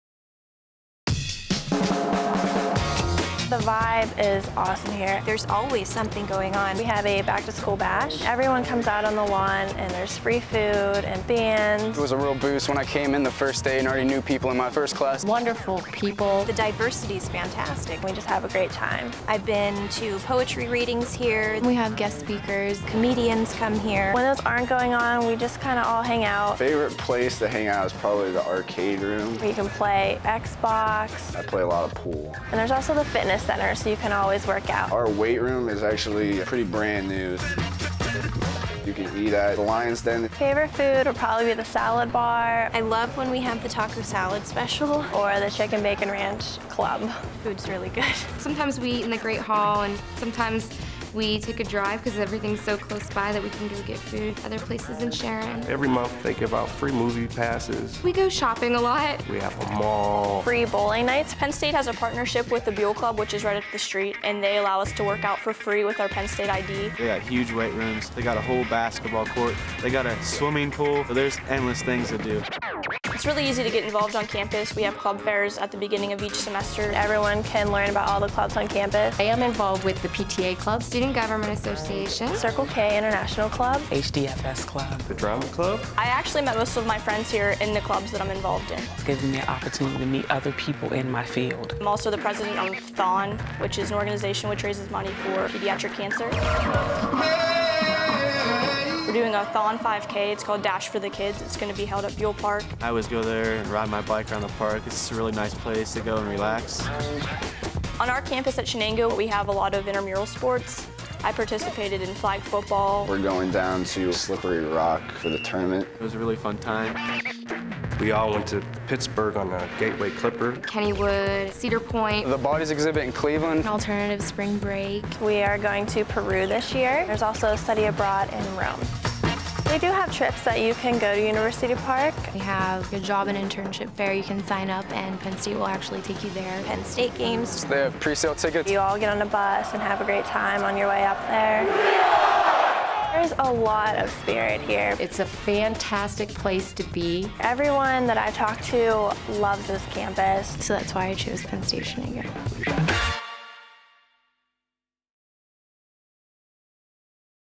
It's Your Time - Shenango - Get Involved
Your college life gives you time to join clubs and organizations, make lifelong friends, enjoy your surroundings, and have fun. You can shoot some pool or play video games in the game room, work out in the fitness center, play sports, go on field trips, participate in alternate spring break to help others, study abroad---those are just a few examples of ways you can get involved at Penn State Shenango.